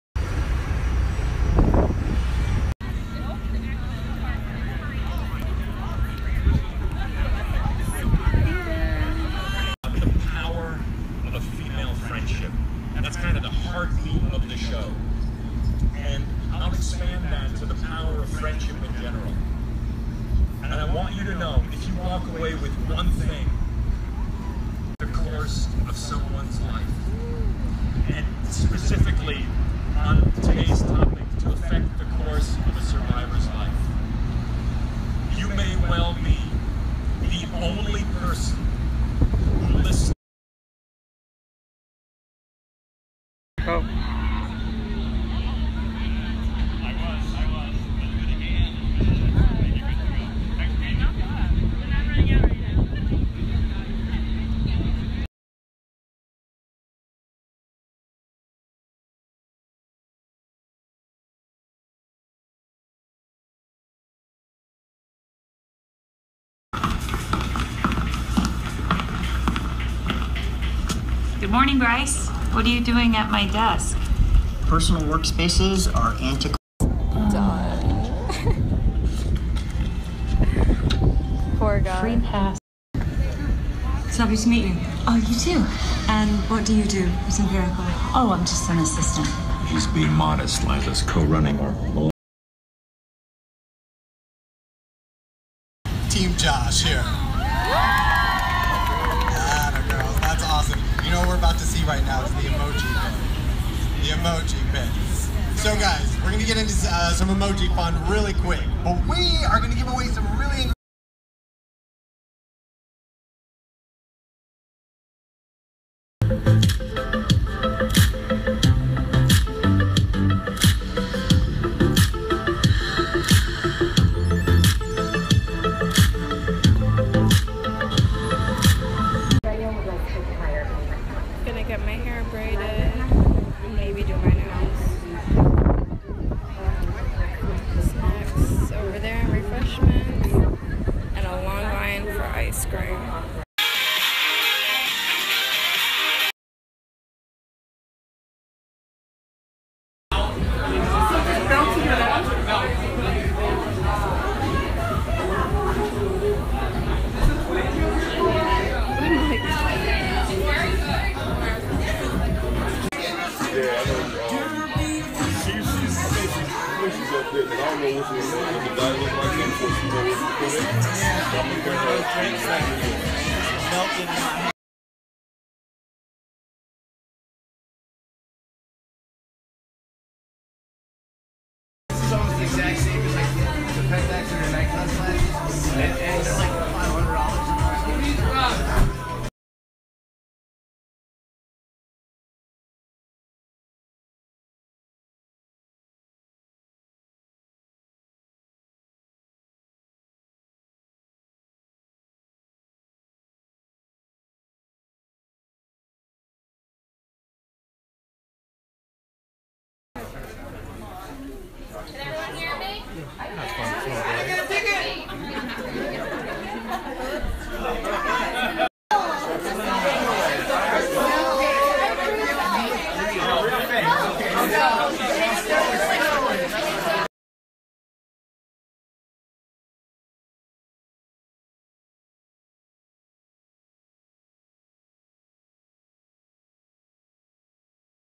GETTING PHOTOBOMBED BY PETER HERMANN & MEETING NICO TORTORELLA YOUNGER & CINNABON EVENT -HD
This vlog is a bit of old clips from the Summer time of when I attended two events one was for the show Younger which was an event for a cause founded by Mariska Hargitay who is known for her role in Law & Order & is also Peter Hermann's wife. He also plays Charles on the show & I also met Nico who plays Josh! The other event I won tickets for from Mel Rose here on YouTube she is also a blogger & I got the chance to meet her which was nice & met some awesome girls. My boyfriend was my plus one & he won a prize!

Etsy Shop

xx Valslifestyle.